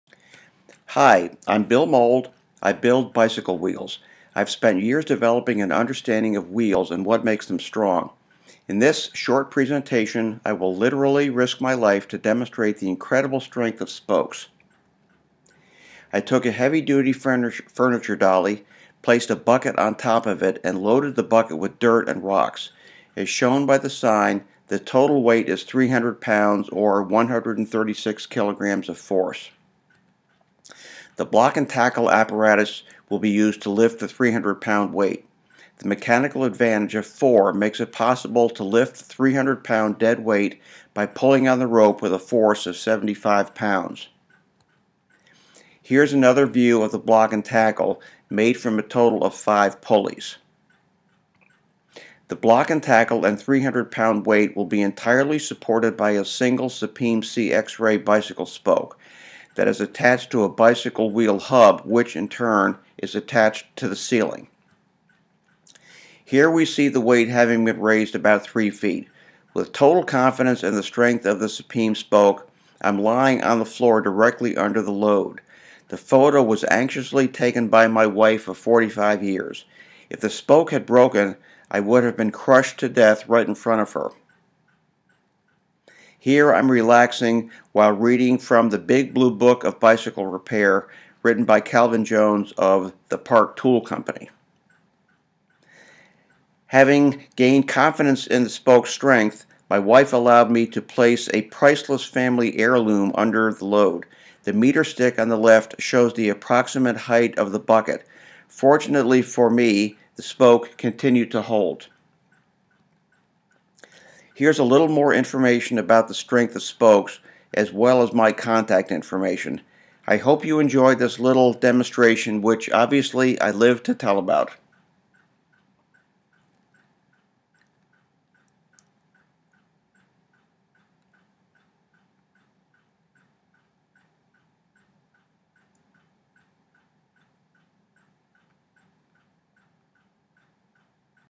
Spoke Strength Death Defying Demonstration
Movie shows a demonstration of the strength of Sapim CX-Ray bicycle spokes in holding up a 300 pound load while it is suspended over Bill Mould.